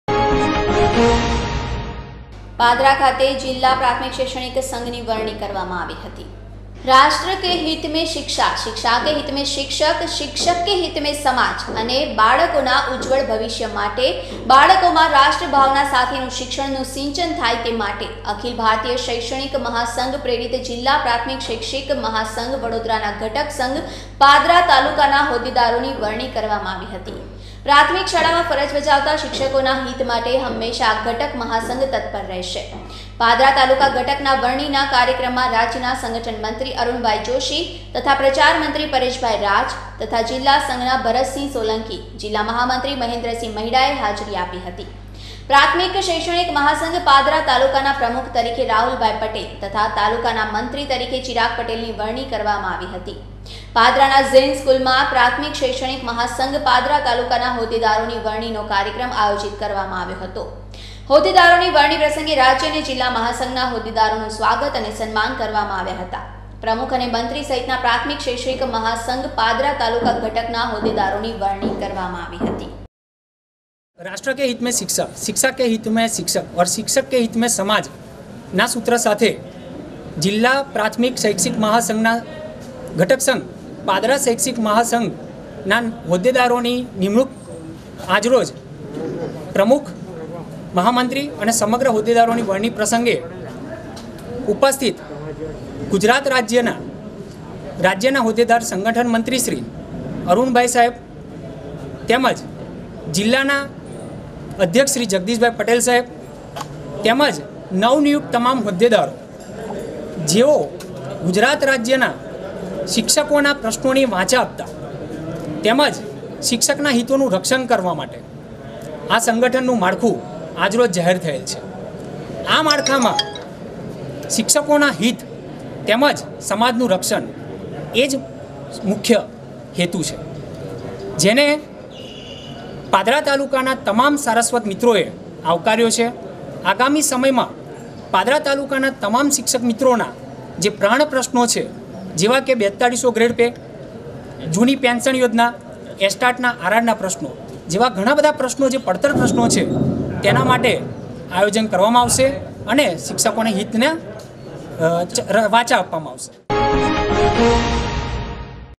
LOKMAT NEWS VADODARA_12-10-20
જિલ્લા પ્રા. શૈક્ષિક મહાસંઘ પાદરા તાલુકાના હોદ્દેદારોની વરણી કરાઈ